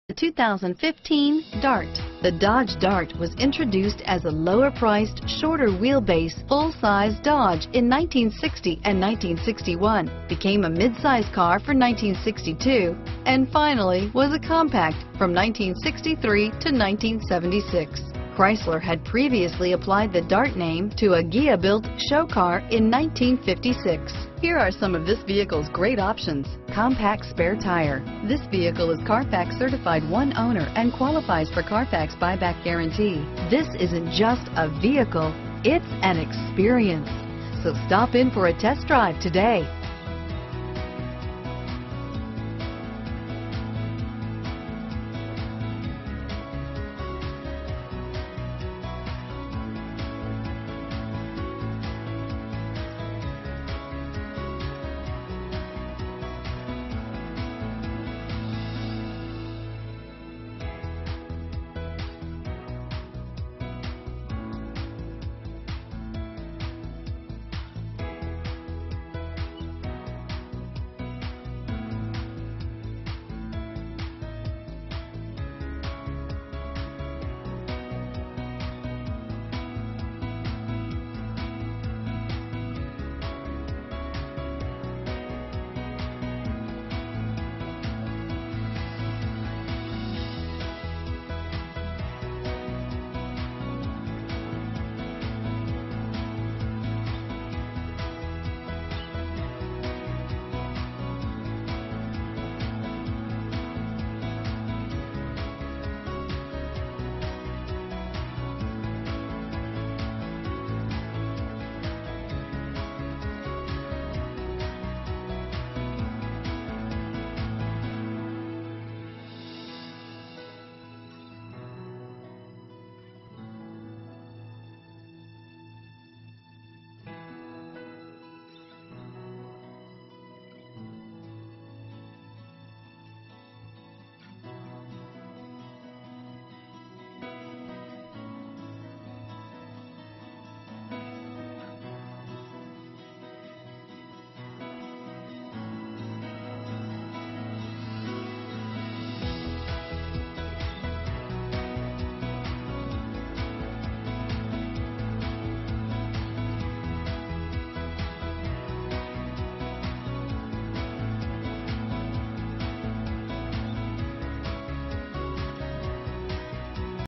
2015 Dodge Dart Atlanta, Duluth, Buford, Roswell, Marietta, GA 146171WP
2015 Dodge Dart

Shottenkirk's Chrysler (Cherokee Chrysler)
200 Liberty BLVD

COMPACT SPARE TIRE,ENGINE: 2.4L I4 MULTIAIR  (STD),GRANITE CRYSTAL METALLIC CLEARCOAT,LIGHT FROST/BLACK  PREMIUM CLOTH SEATS,MANUFACTURER'S STATEMENT OF ORIGIN,QUICK ORDER PACKAGE 28B  -inc: Engine: 2.4L I4 MultiAir  Transmission: 6-Speed Automatic,TIRES: P205/55R16 AS  (STD),TRANSMISSION: 6-SPEED AUTOMATIC  -inc: Urethane Shift Knob  Underbody Aerodynamic Treatment  Autostick Automatic Transmission  Active Grille Shutters  Tip Start,WHEELS: 16 X 7.0 MONOTONE PAINTED ALUMINUM  (STD),Radio: Uconnect 200 AM/FM/CD/MP3,Uconnect w/Bluetooth Wireless Phone Connectivity,4-Wheel Disc Brakes w/4-Wheel ABS  Front Vented Discs  Brake Assist and Hill Hold Control,Tire Specific Low Tire Pressure Warning